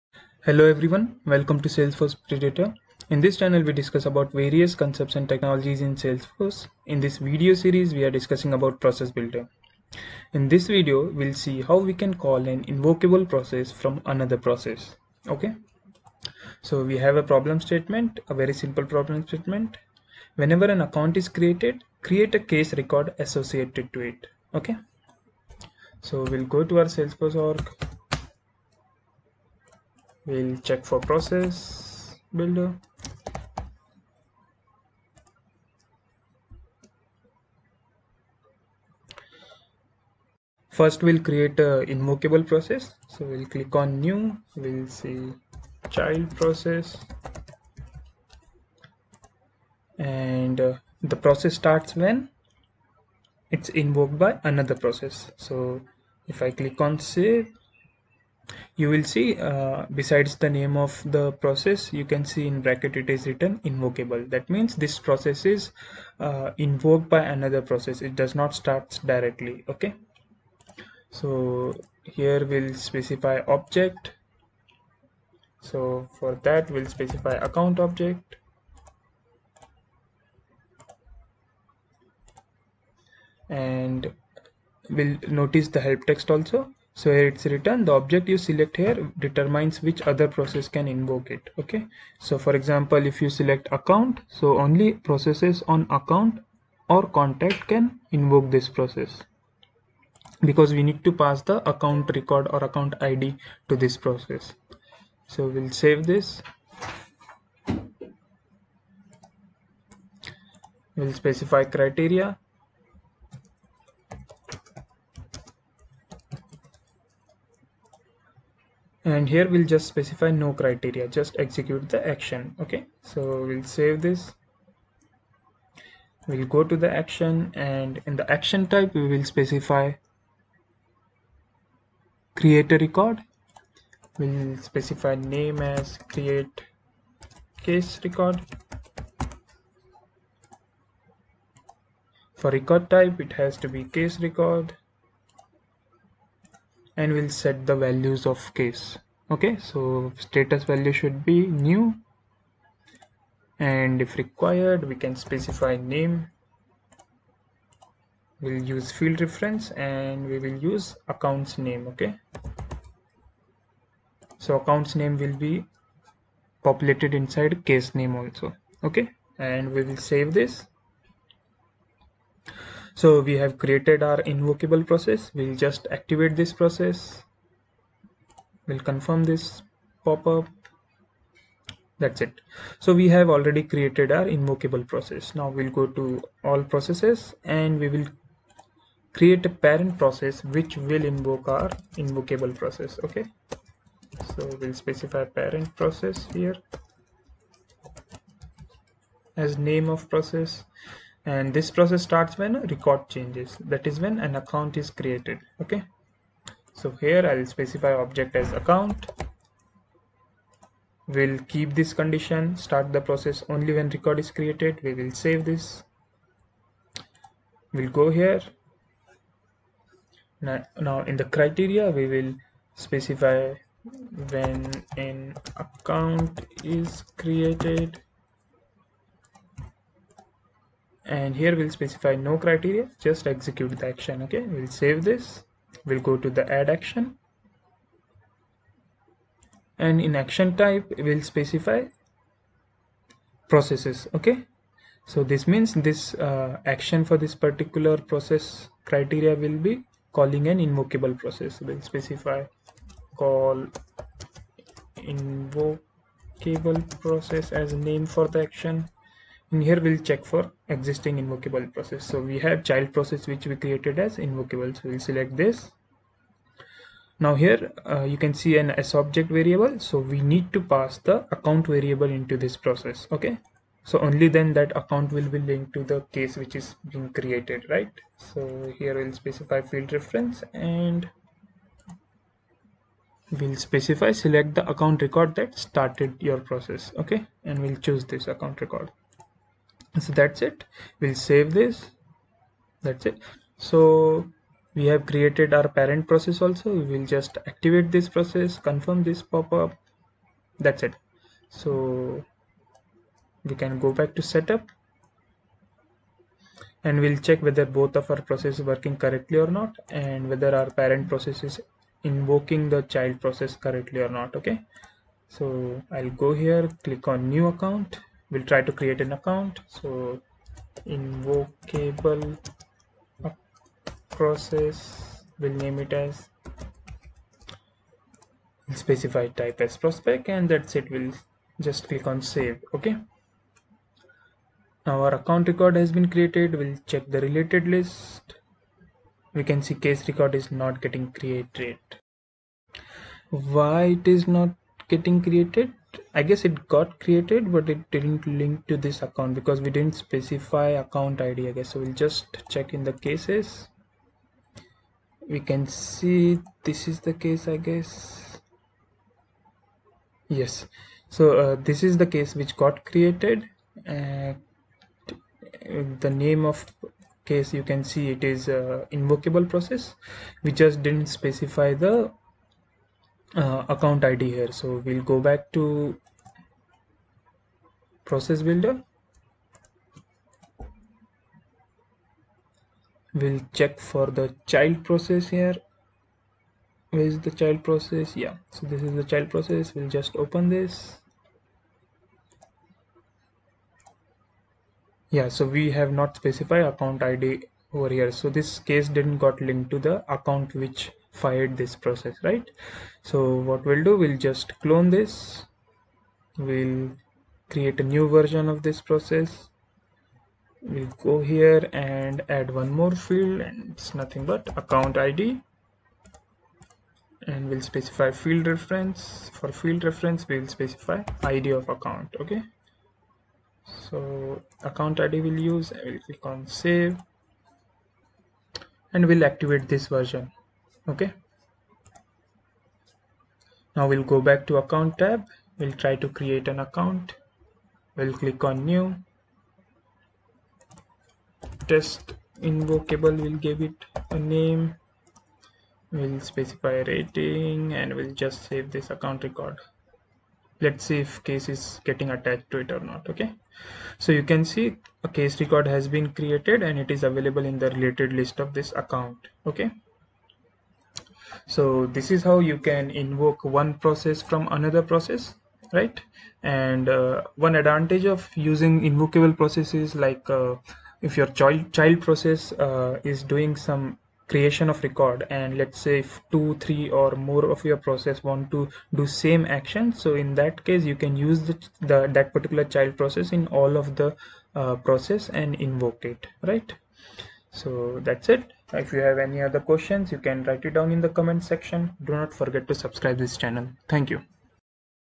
Process Builder in Salesforce : Calling Invocable Process from Another Process Using Process Builder
In this video, we discuss how we can call an invocable process from another process in salesforce.
We see how we can start a process when invoked by another process.

and get best discounts on the courses.